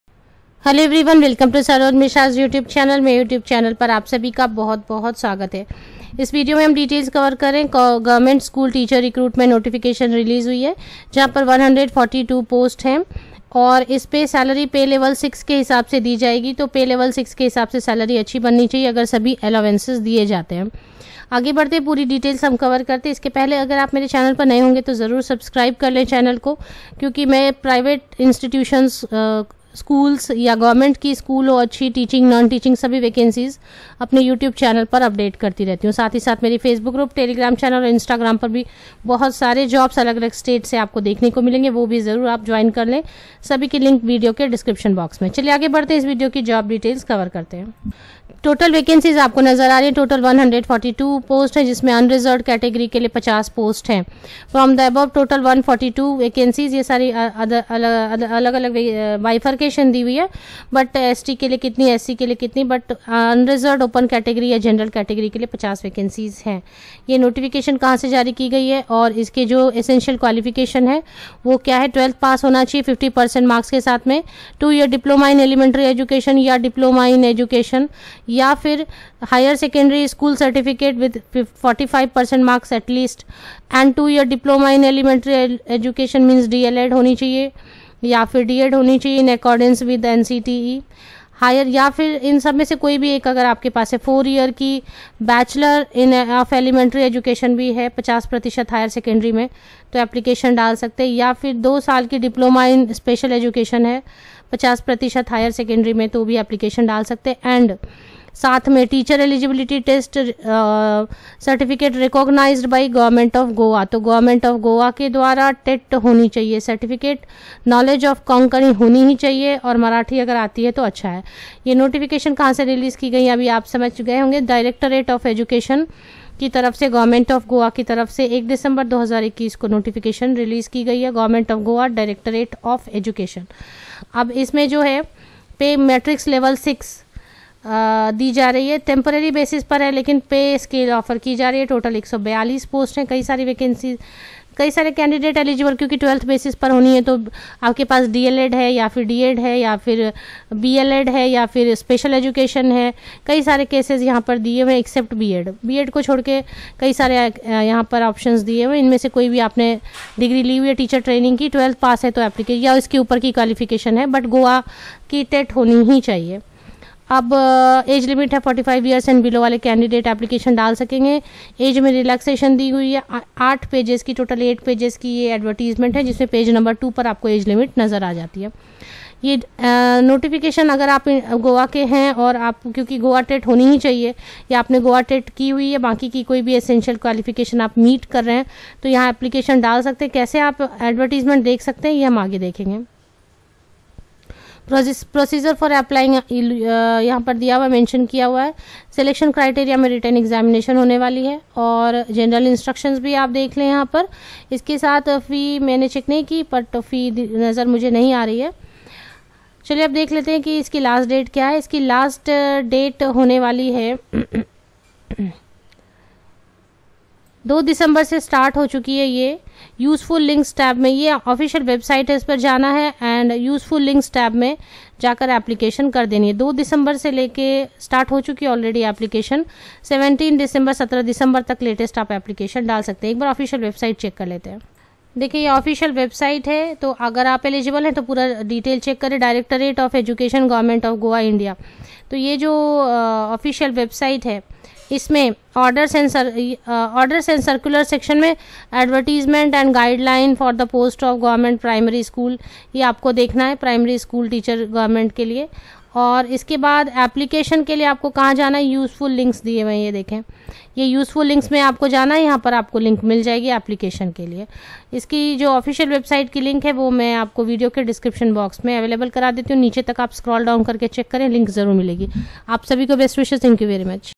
142 GOVT TEACHERS POSTS I 52000 Rs SALARY PM I GOVT SCHOOL TEACHERS RECRUITMENT I APPLY TILL 17 DEC
142 GOVT TEACHERS POSTS I 52000 Rs SALARY PM I GOVT SCHOOL TEACHERS RECRUITMENT I APPLY TILL 17 DECEMBER, 2021 III

BELOW IS OFFICIAL WEBSITE LINK FOR DETAILS AND ONLINE APPLICATION